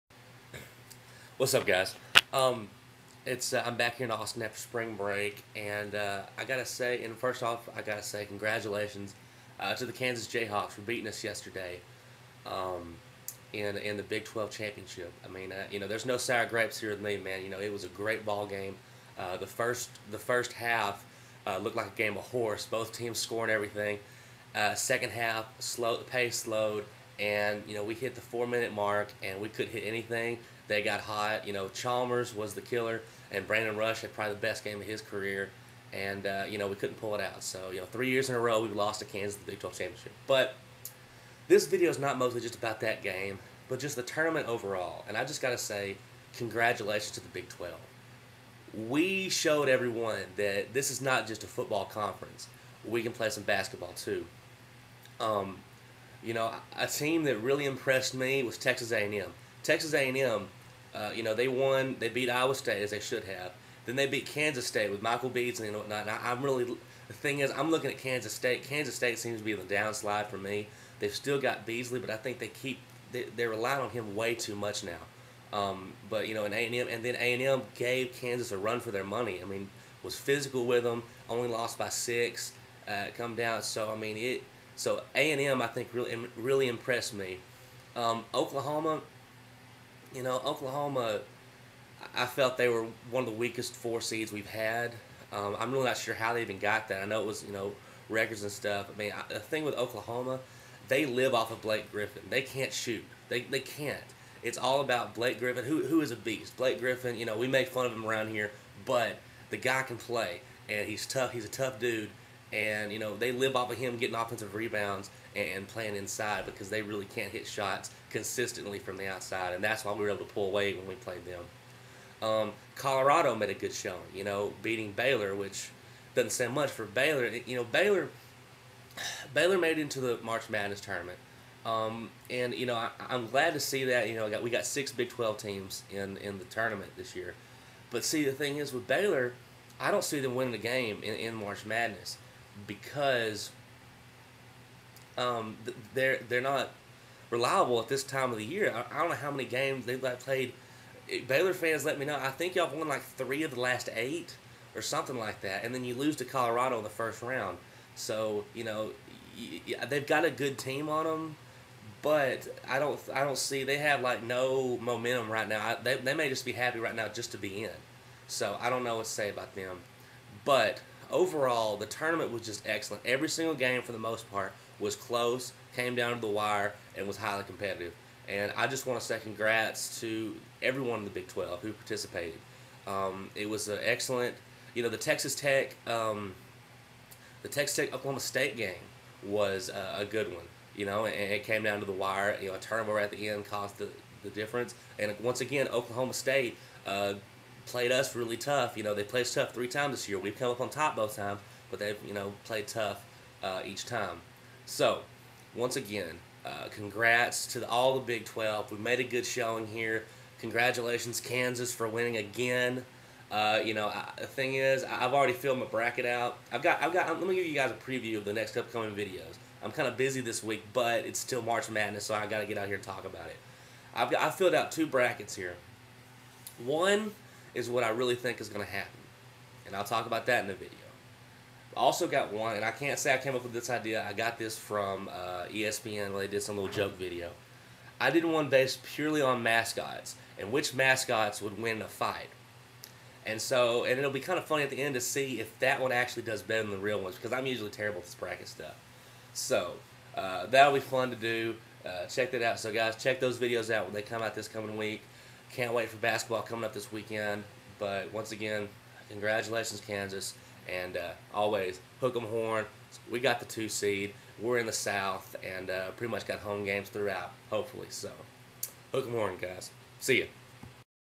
Texas Fan Reaction to Big 12 Tournament
Congratulations to the Kansas Jayhawks for winning the title, but also congrats to the entire Big 12 conference for putting on a great competitive Tournament.